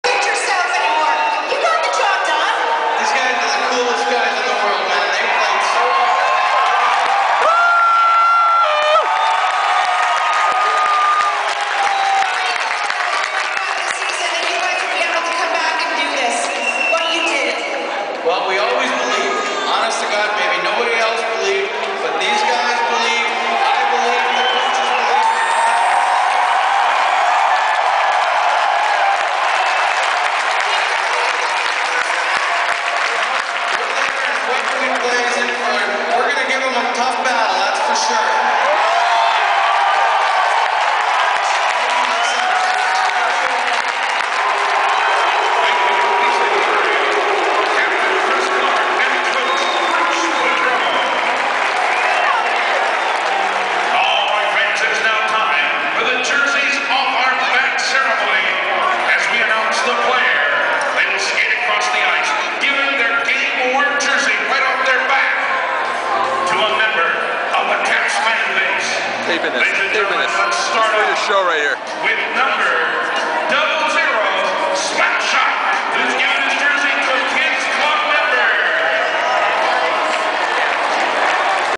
Caps Clinch Division Title!
Coach Bruce Boudreau addresses the faithful at the phonebooth following the Caps 3-1 win over the Panthers to clinch a playoff berth.